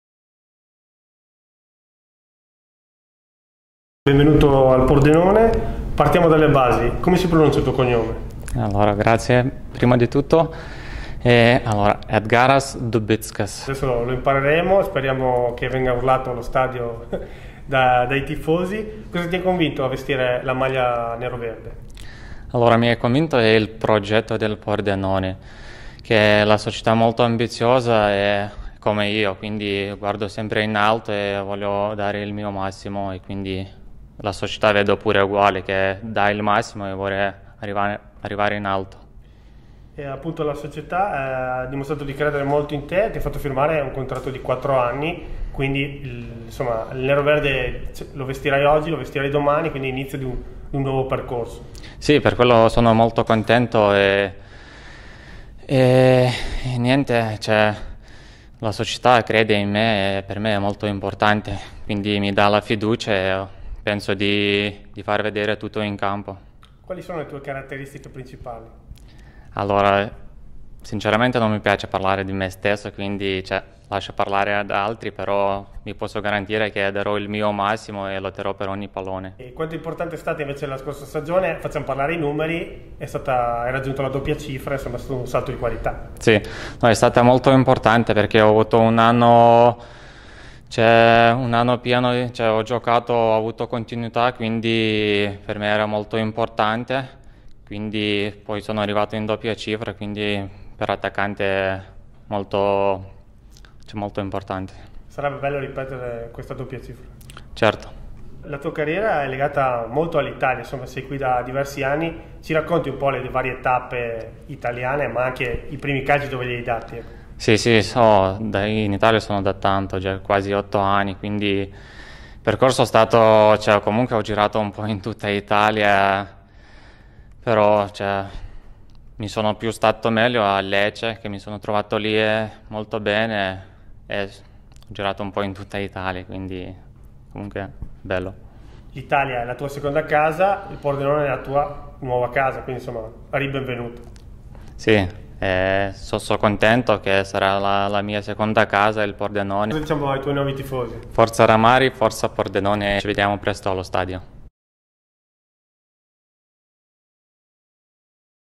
LE PRIME PAROLE DI EDGARAS DUBICKAS IN NEROVERDE
"Sono molto felice di essere qui e non vedo l'ora di iniziare. La Società ha dimostrato di credere fortemente nelle mie qualità, ho firmato per 4 anni e voglio dare il massimo in campo".

Edgaras Dubickas si presenta al mondo neroverde.